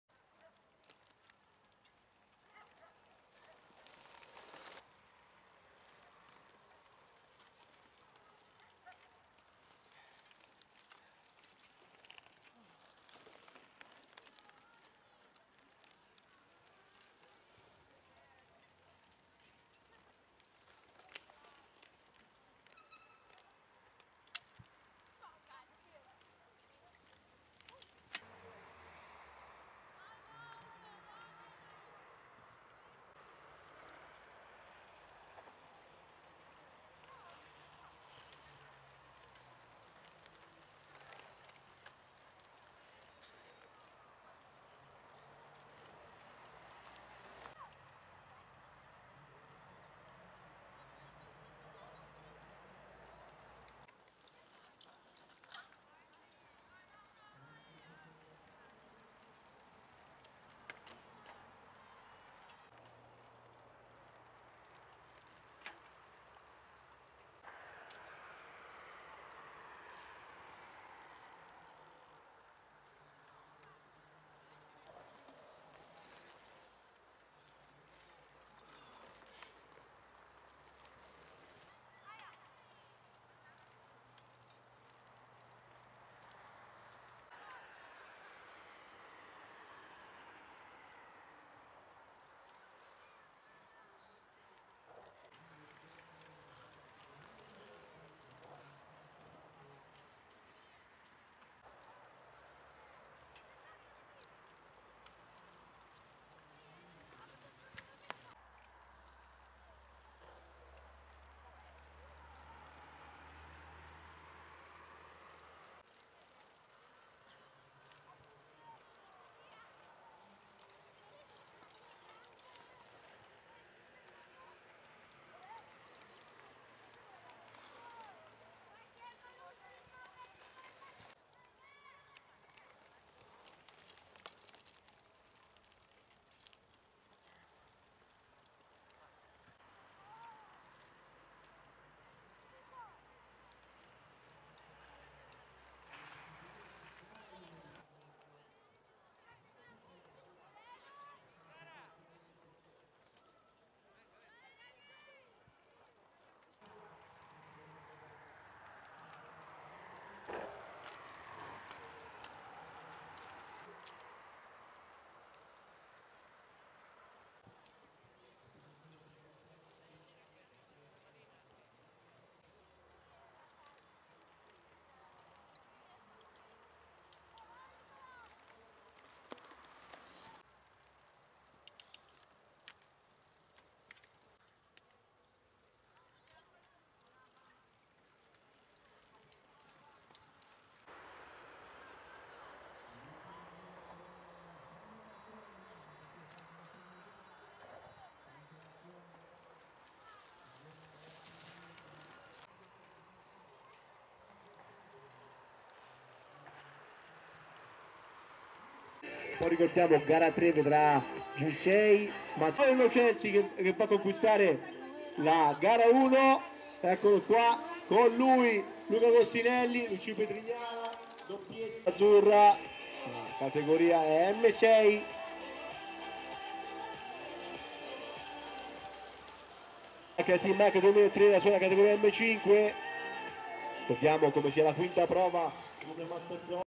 Cerqueto Cross 2016 - gara1 pt2
Quinta prova dell'Umbria Master Cross 2017.
Organizza ASD Alfredo Comodi.
Memorial Anna e Ernesto Spigarelli